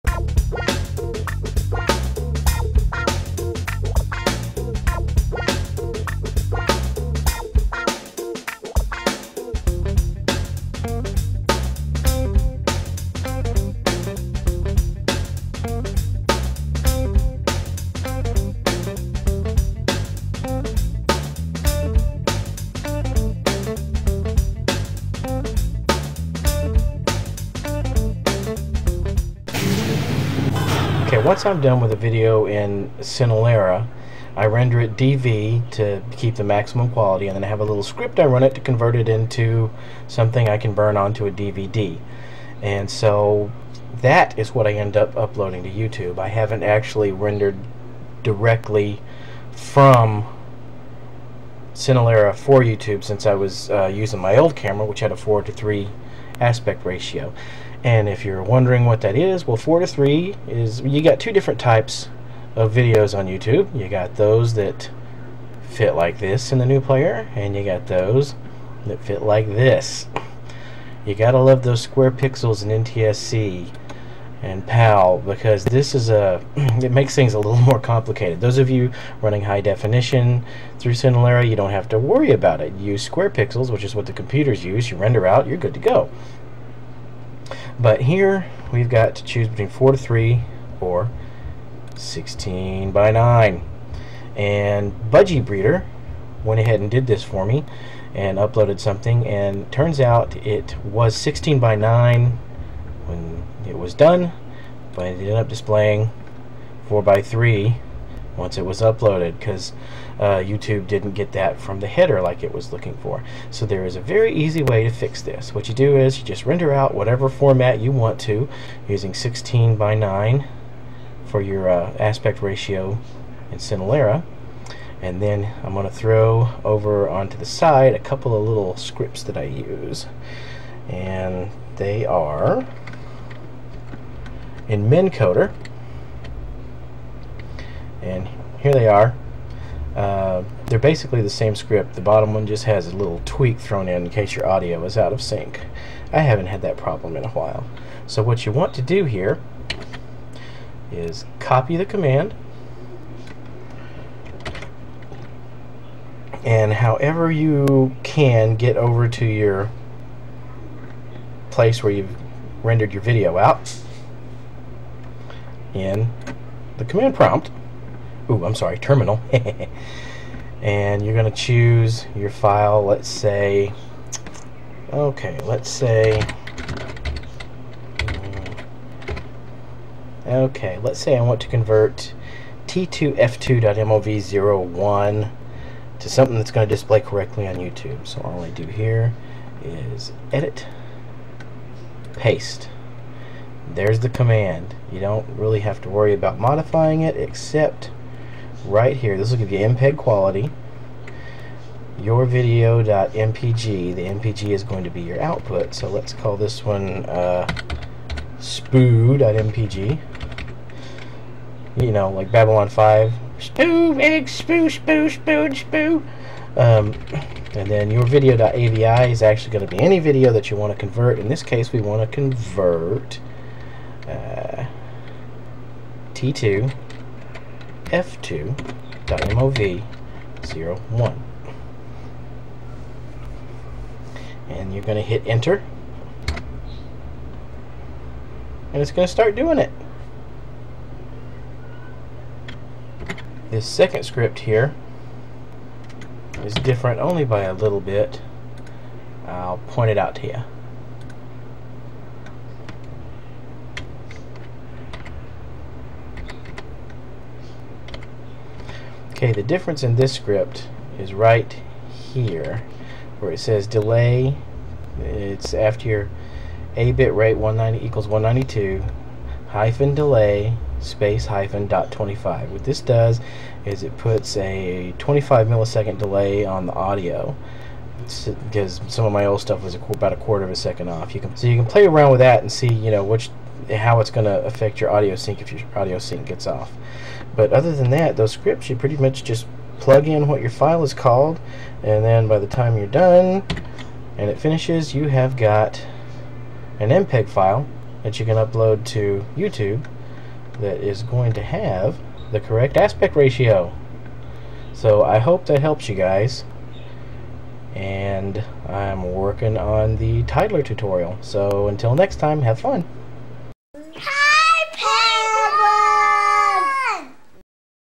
Cinelerra: Getting 720x480 Widescreen (16:9) for YouTube
Thanks to budgiebreder for trying this out for us - if you're rendering out 720x480 16:9, YouTube apparently doesn't see the aspect ratio information and encodes it at 4:3.  Here's a little command (hopefully you got mencoder based on my previous videos - if not, "sudo apt-get install mencoder" will get it from Ubuntu) that will output a DVD quality file at 16:9, ready for YouTube.  In fact... I used it for this!  Here's the scripts... And yes, I was feeling bump deprived!

mencoder -oac lavc -ovc lavc -of mpeg -mpegopts format=dvd -vf scale=720:480,harddup -srate 48000 -af lavcresample=48000 -lavcopts vcodec=mpeg2video:vrc_buf_size=1835:vrc_maxrate=9800:vbitrate=5000:keyint=18:aspect=16/9:acodec=ac3:abitrate=192 -ofps 30000/1001 -o your_video.mpg your_video.***

mencoder -oac lavc -ovc lavc -of mpeg -mpegopts format=dvd -vf scale=720:480,harddup -srate 48000 -af lavcresample=48000 -lavcopts vcodec=mpeg2video:vrc_buf_size=1835:vrc_maxrate=9800:vbitrate=5000:keyint=18:aspect=16/9:acodec=ac3:abitrate=192 -delay -.25 -ofps 30000/1001 -o your_video.mpg your_video.***